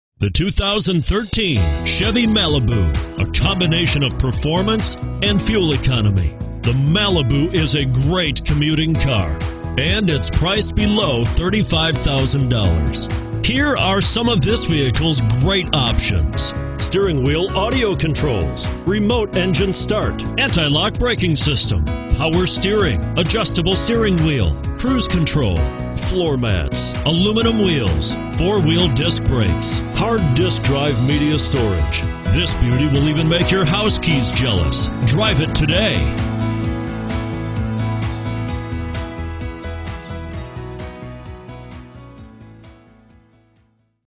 2013 CHEVROLET MALIBU Sutton, WV 50015744
2013 CHEVROLET MALIBU Sutton, WV
Stock# 50015744

Mid-State Chevrolet
1947 Sutton Lane

Pick up this Black Granite Metallic 2013 Chevrolet Malibu, available today. Featuring Automatic transmission, it has 0 miles. This could be the one you've been searching for.